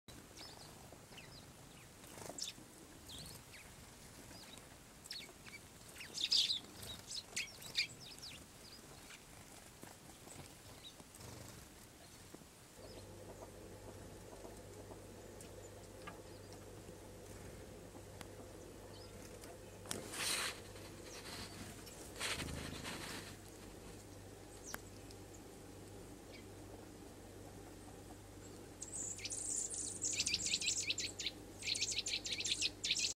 Birds eating in winter
Tree sparrows and blue tits eating fat balls.